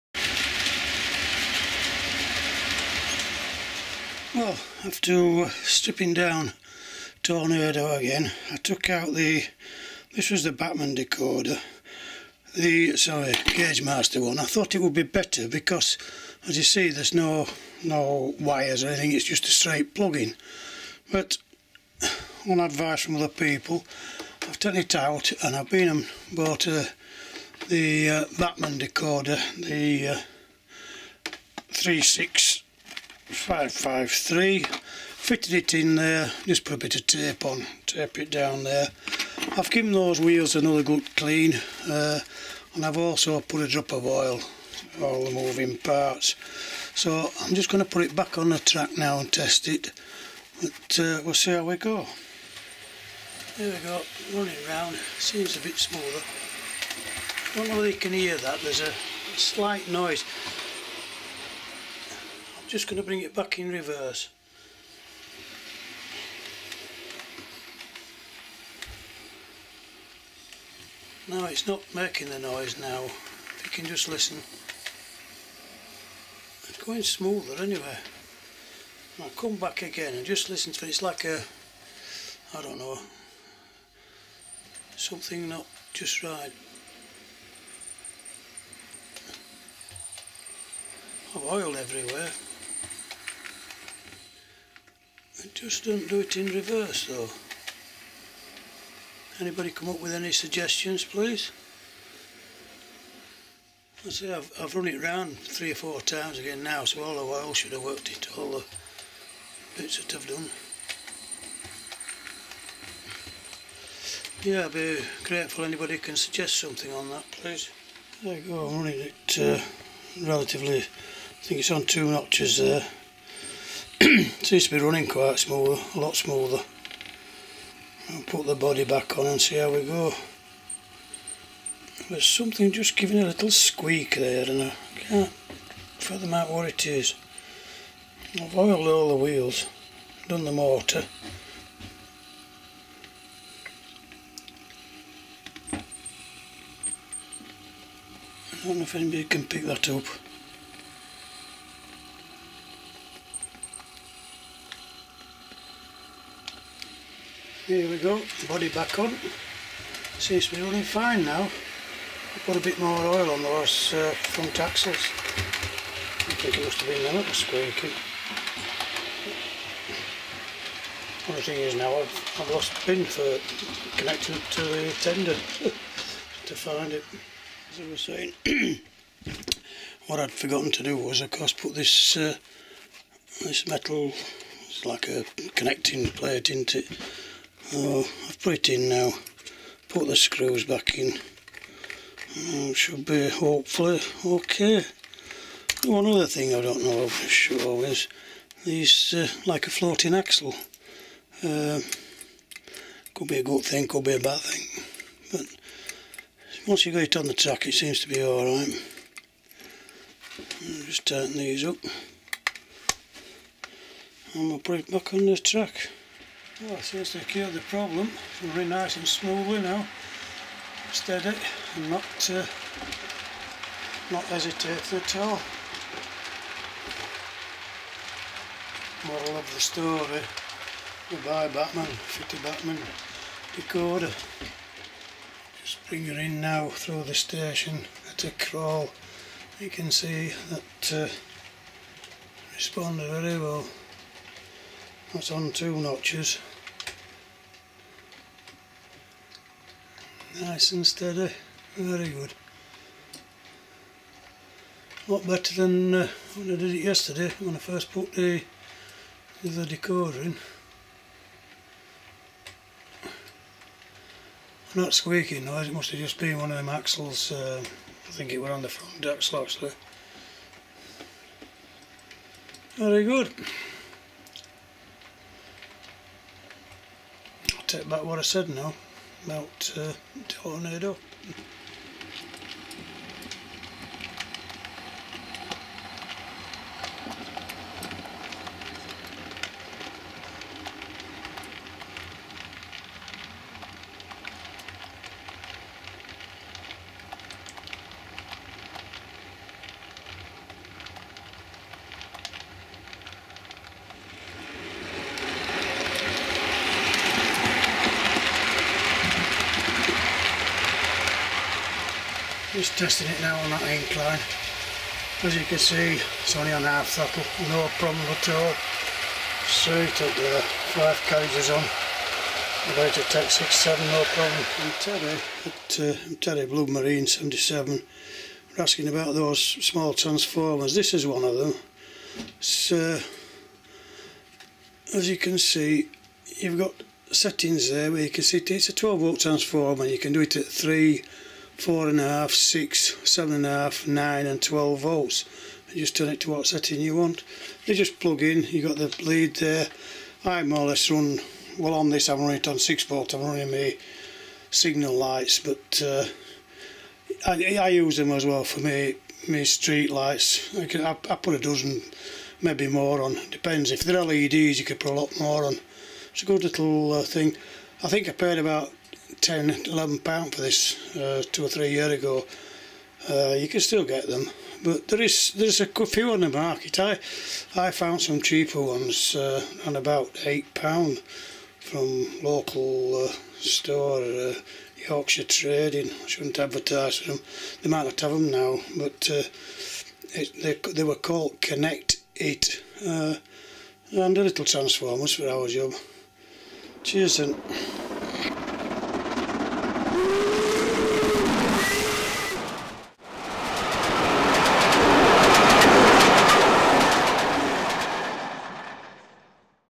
TORNADO FROM BACHMANN PART 2
Produced with CyberLink PowerDirector 11  WE HAVE NOW FITTED THE NEW DECODER AND DONE SOME WHEEL CLEANING AND LUBRICATION , THE RESULT IS A BETTER RUNNING LOCO   the Transformer at the end is a Power Pax